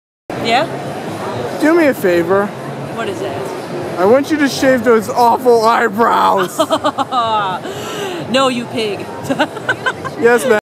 Satsuki at AX2015
I ask about her eye brows.
By the way Ryuko is cool and badass, but Satsuki has class and is best character.